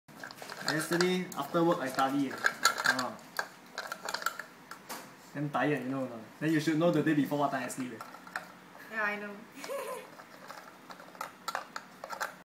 Ruby playing with Ice
My friend's Shetland puppy playing with ice.